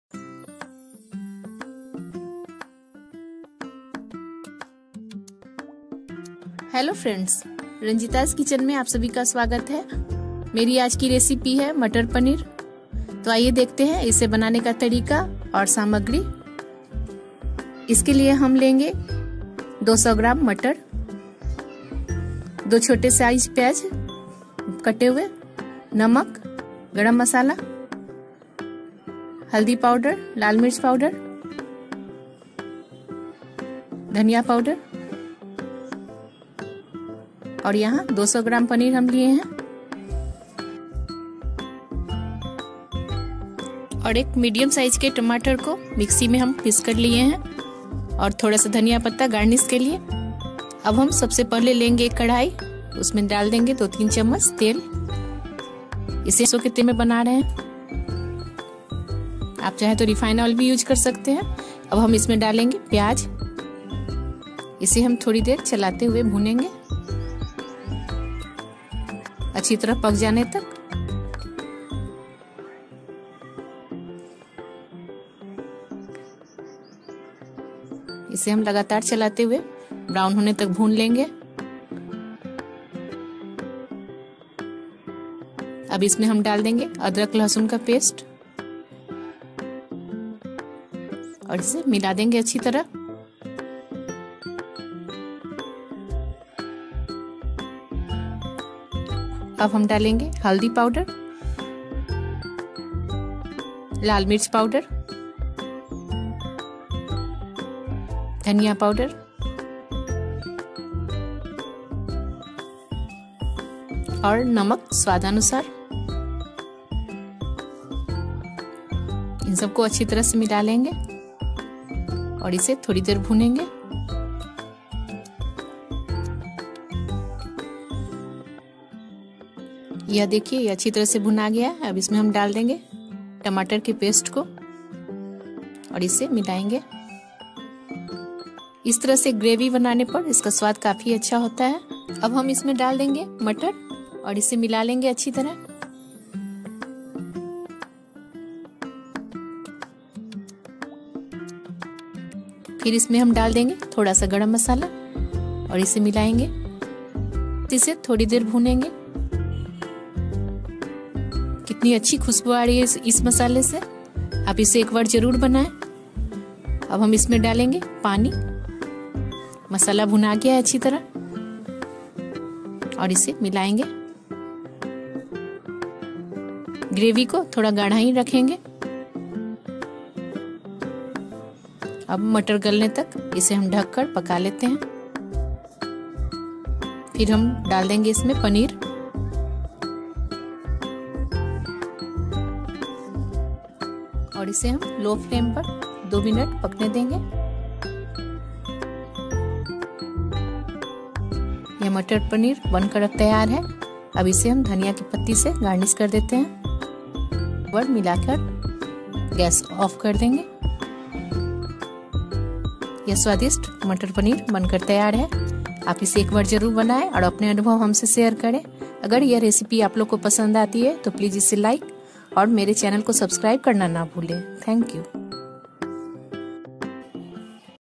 मटर पनीर की सब्जी कैसे बनाए-How to make matar paneer-Matar paneer recipe | Ranjeeta kumari
Hello everyone...!!!

welcome to Ranjeeta's kitchen.

About this video:- Find out how to make tasty matar paneer sabji at your home...Recipe shown in this video is very easy and tasty.

note:- ginger garlic paste is not shown among ingredients.